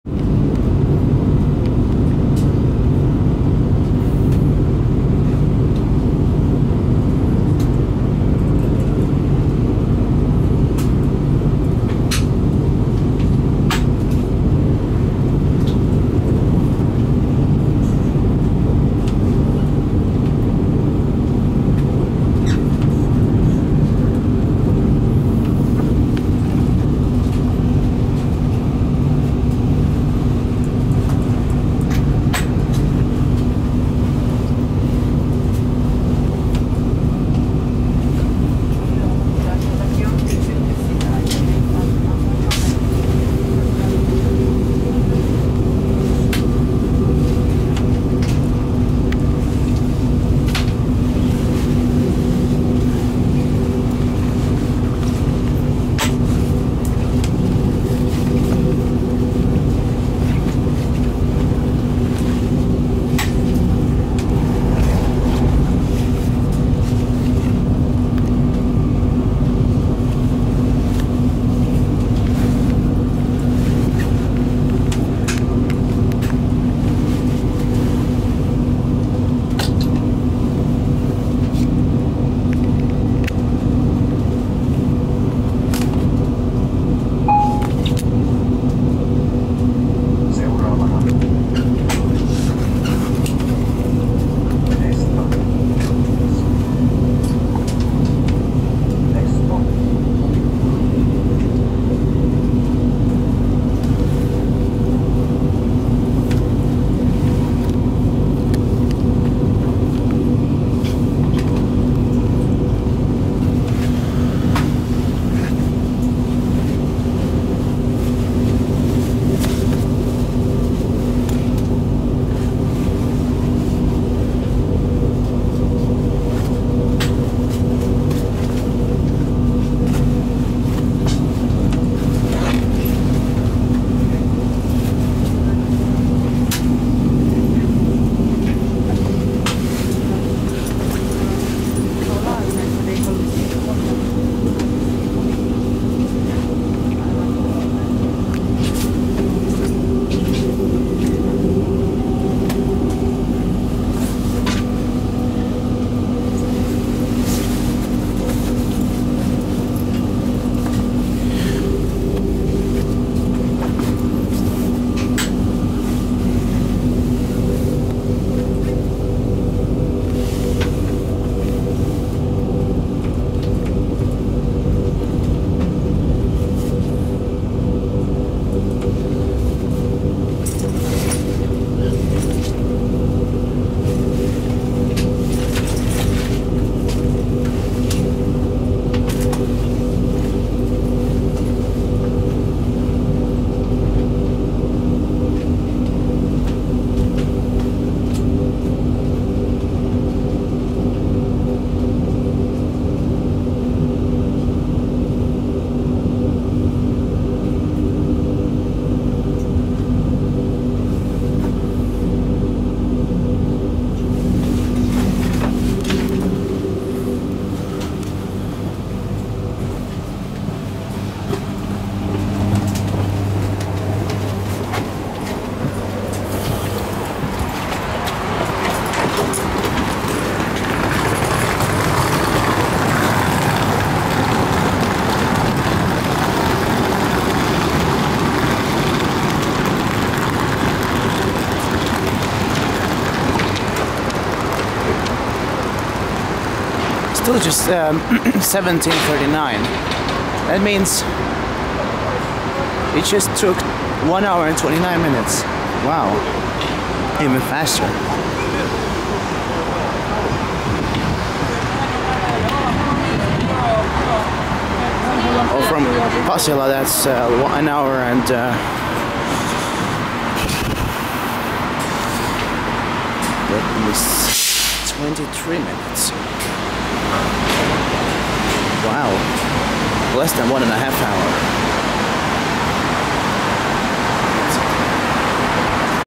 VR Pendolino 975 arriving @ Kupittaa (Kuppis) railway station, 06.04.16, 16:16-17:39, Part 5
This is a video of me riding a Pendolino high-speed train operated by VR, from Helsinki to Turku (stopping only at Pasila and Kupittaa in between) and getting off at Kupittaa, on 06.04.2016, at 16:16-17.39.This Pendolino train was manufactured by Alstom in 2000.

VR recently introduced two high-speed train services on the Turku-Helsinki line (coastal line - Rantarata, Kustbanan) with the Pendolino trains, reducing travel time by 20 minutes (from 1h58min to 1h38min) on the 193,0 km long railway line. The trains can reach speeds of 220 km/h, but currently, a maximum of 200 km/h is allowed on some of the parts of the Coastal Line. VR just updated the tilt system in its Pendolino inter-city trains and has also worked with the Finnish Transport Agency to remove speed limits on five sections of track to speed up the trip between the two cities.

Now it only takes an hour and 38 minutes from Turku central railway station (193,0 km) to Helsinki central railway station (0km). For me, it is more convenient to go from Kupittaa railway station (189,7 km) after visiting my family, so then it's 5 minutes less - just about an hour and a half! This new service only stops at Kupittaa and Pasila (3,2km) (1h27min or 1h26min back), skipping all the other stations (and it's 186,5km on rail between Kupittaa and Pasila!), and it is intended for commuters from Turku to Helsinki and therefore, the schedule on weekdays (no weekend services) is as follows: Turku-Helsinki 7:08 - 8:46 (Kupittaa 7:13, Pasila 8:40). Helsinki-Turku 16:10-17:48 (Pasila 16:16, Kupittaa 17:42). We were even faster than our schedule today (3 minutes faster!), and it took only 1:29 after we started (Pasila-Kupittaa 1:23!), and it was pretty fast - much faster than the InterCity trains! Now, the government of Finland has given 40 million euros for the detailed planning of the coming "One hour train" project, and the planning will take 3 years, until the decicion about the construction of it will be made, which would reduce travel time by at least another 20 minutes between Turku and Helsinki (40km shorter track - down to just 153km in total, including 75km new track from Salo via Lohja (Lojo) to Espoo) for the high-speed trains (Kupittaa-Pasila in exactly one hour) and keeping the old track from Salo via Karis, Inkoo, Siuntio, Kirkkonummi to Espoo for the local trains! The tickets aren't more expensive for the Pendolino trains on this service. VR recently decreased the prices (student tickets now 12-14€, before it was 16-21€, adult tickets now 20€, before it was 28-35€), and also, the cheap discount tickets for these trains can also be found if you purchase the tickets about 2-3 days in advance, sometimes longer (only 9,90 euros for students and adults!), further challenging the market of the slow cheap buses for 5-10 euros (these trains are now more than half an hour faster than the buses because the Turku-Helsinki buses all take at least 2h and 5mins), also, I prefer rail transport because it's much more convenient, it's faster and it's also ecological.

I filmed the whole train ride in 5 parts.